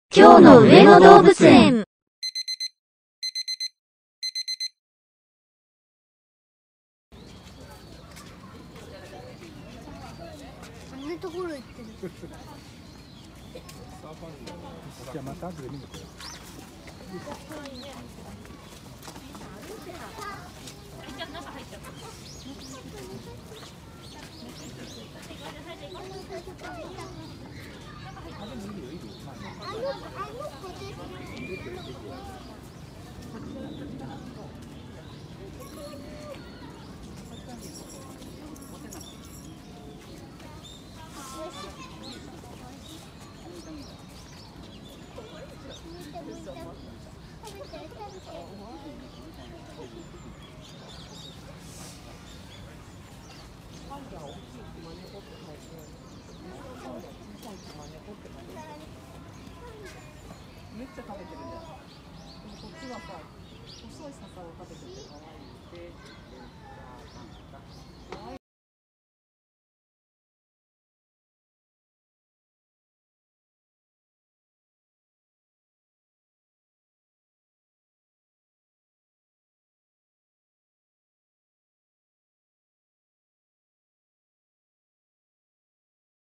カリンお母さん、ルカちゃん のようすです。【2023年1月7日】【上野動物園】レッサーパンダ
【2023年1月7日】【上野動物園】レッサーパンダの様子です